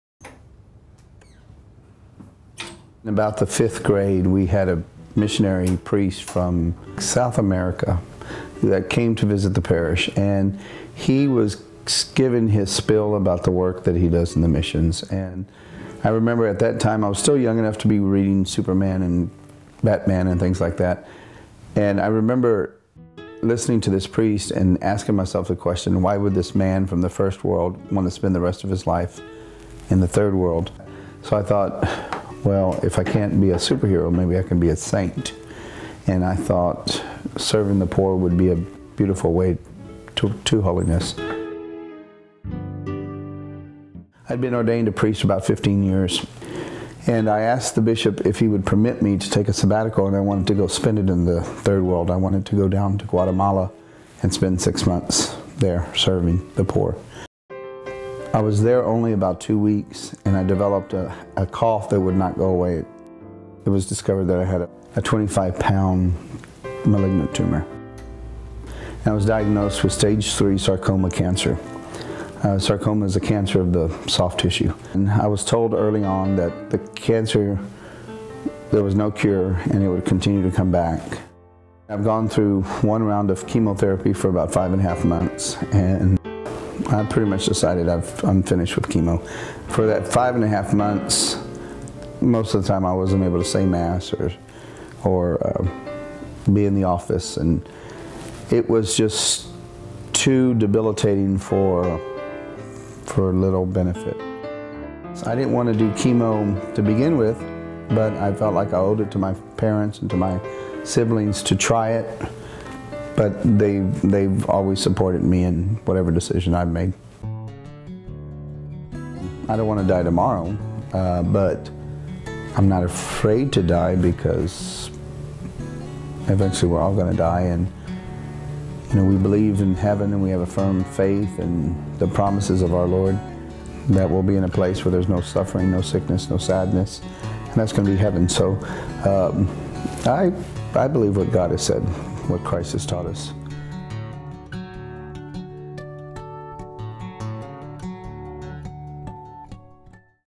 Fr. Dan Morales: An Answered Call
Fr. Dan Morales is a Catholic priest in Victoria, Texas. He recounts how he discovered what his vocation is and the journey it has led him on.